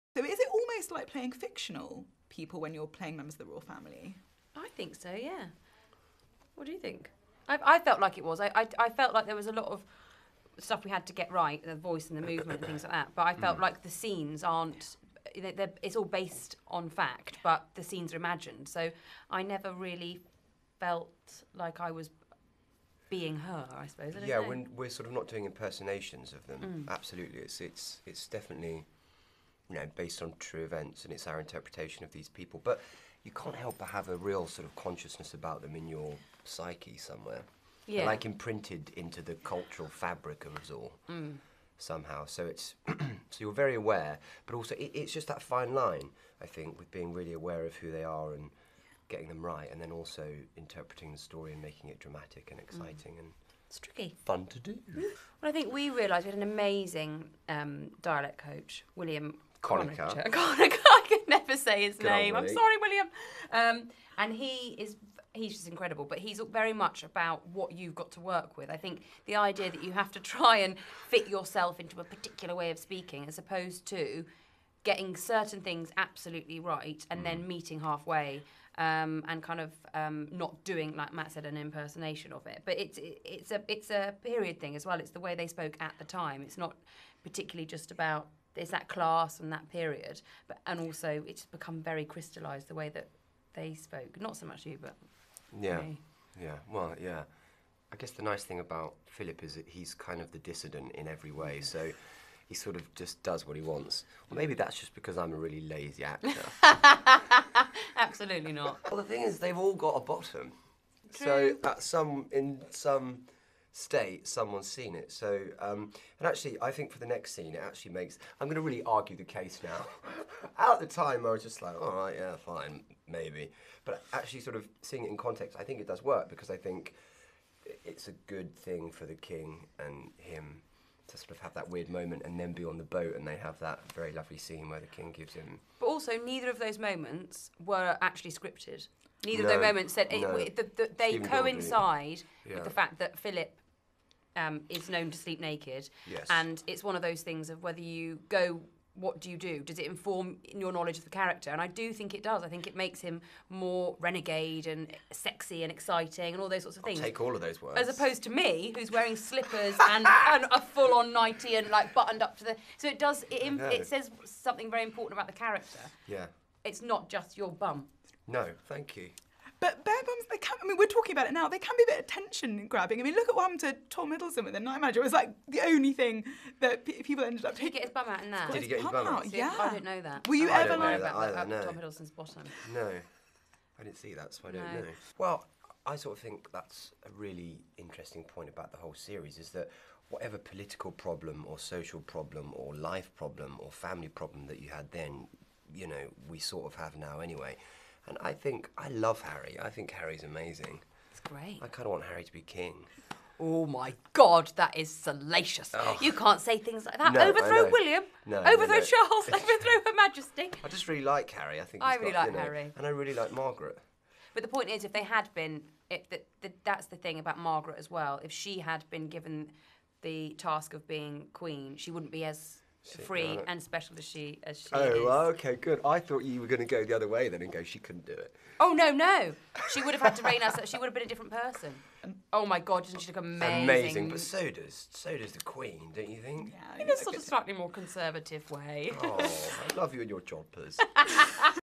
Matt Smith struggled with Prince Philips accent in The Crown - Daily Mail
It's one of the most highly-anticipated shows of the season. So it came as no surprise to see that Matt Smith and Claire Foy (centre) were in chirpy spirits as they walked the red carpet at The Crown premiere held at Leicester Square's Odeon Cinema on Tuesday. Cuddling up together, the stars seemed keen to promote their new Netflix series, but it was the show's corgi (inset) that became the unlikely star of the evening as it followed on behind its human co-stars. The lovebirds were accompanied by actresses Gillian Anderson (right) and Vanessa Kirby (left), who turned heads in their glamorous dresses.